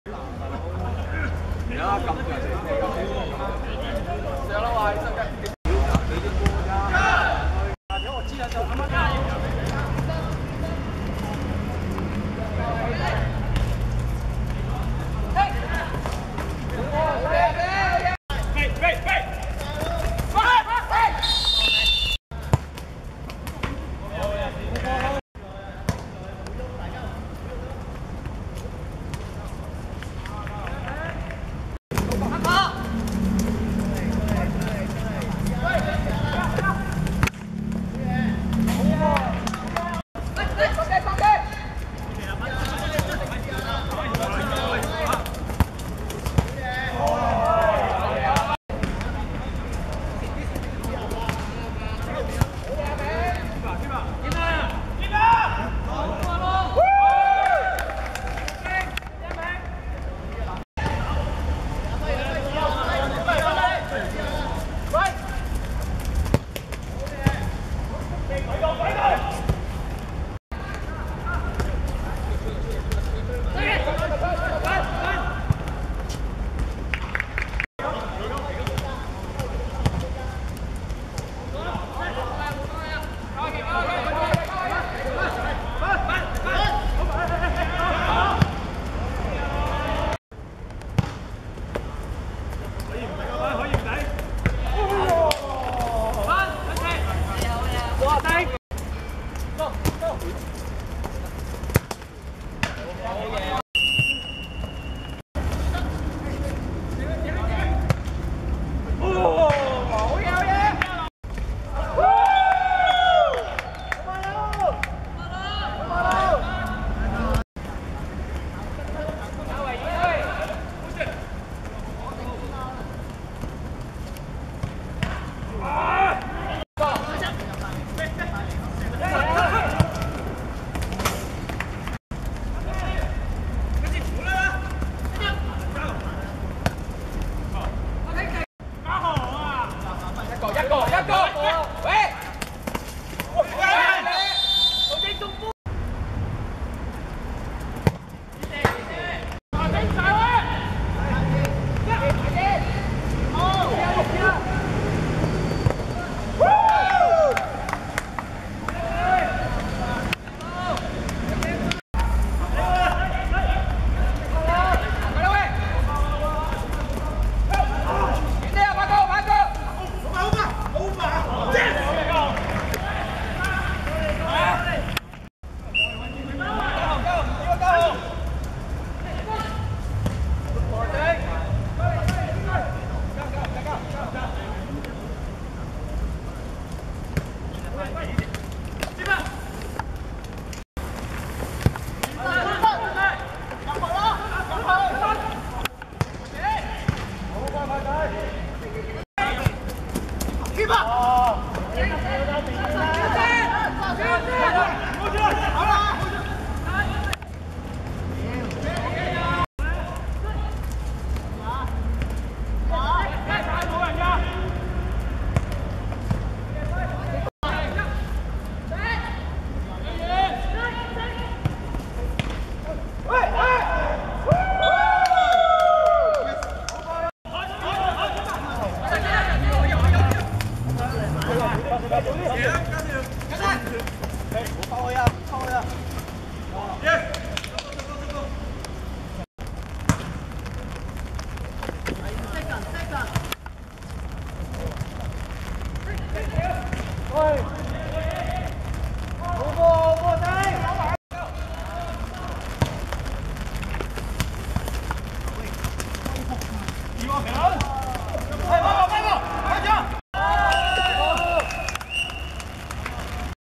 恐龍戰隊vs天柏(2015.4.10.新界西足球銀碟賽四強)精華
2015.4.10.青田遊樂塲 22:00 恐龍戰隊(黃) 4:2 天柏(紅)